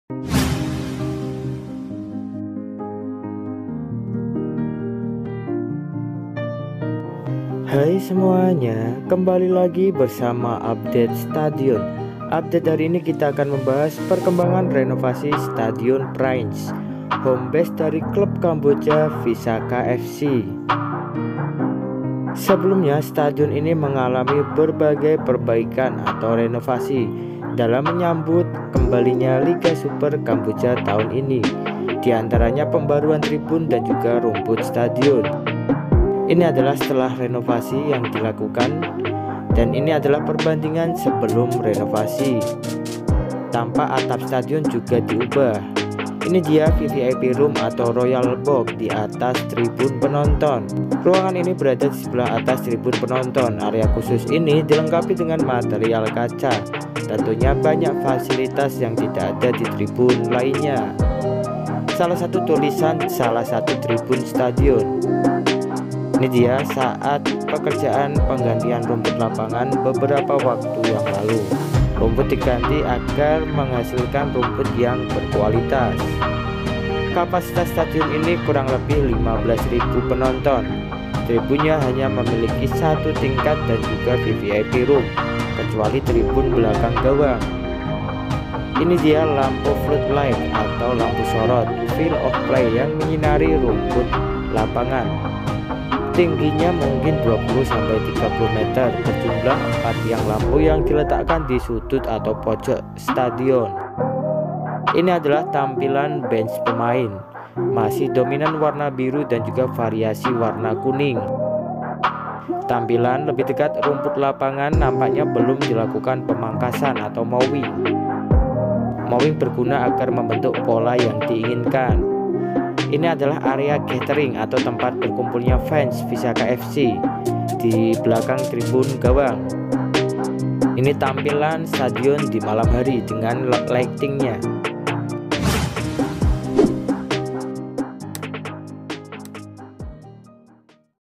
SIMPLE TAPI BERKELAS! Update Renovasi Stadion Visakha FC (Kamboja)! Single Seat, Exterior, Rumput,
Update Stadion Visakha FC